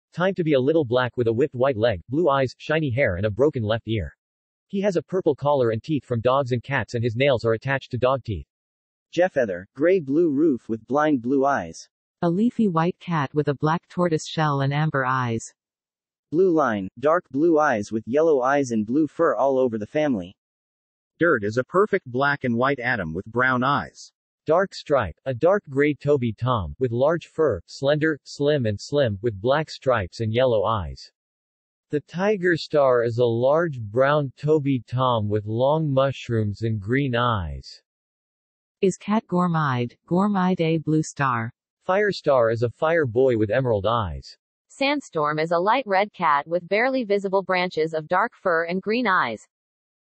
Warrior Cats According to Google Translate
I still feel horrible from covid, but whatever.

This video was inspired by Northflowo's video! (Obviously)

Cats (In order of appearance):
Scourge
Jayfeather
Spottedleaf
Bluestar
Smudge
Darkstripe
Tigerstar
Supposed to be Graystripe
Firestar
Sandstorm

this is dumb lol